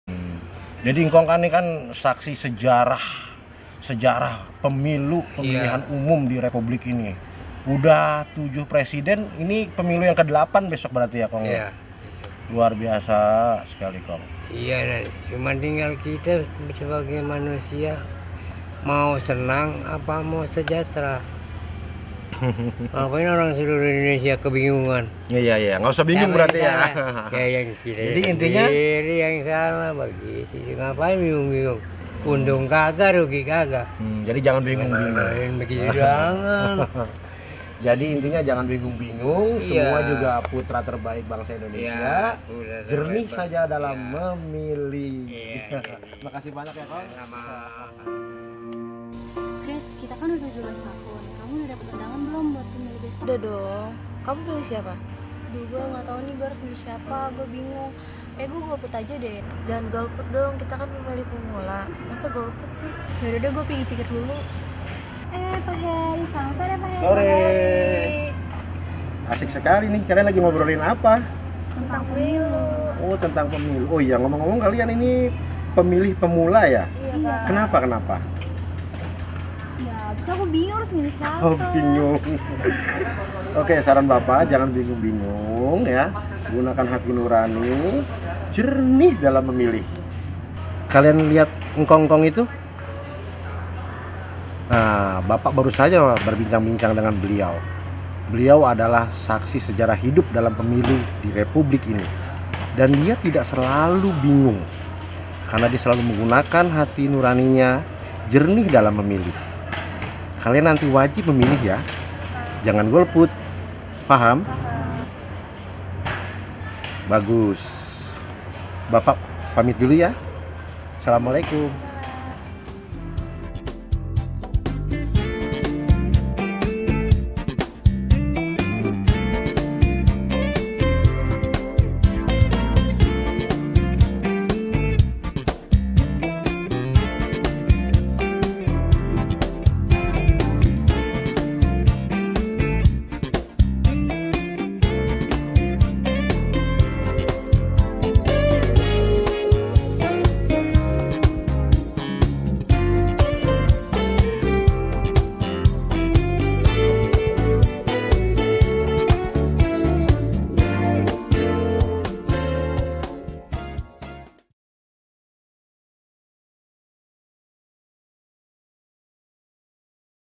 KUMPARAN "Jernih memilih bagi pemilih pemula" | Short Movie
obrolan seorang guru smk dengan engkong sadar sebagai saksi hidup sejarah pemilu di negri ini yg selalu jernih dalam memilih. sementara di taman sekolah ada dua siswi yg sedang galau tentang pemilih pemula yg bingung dalam pemilu  17 April 2019 nanti.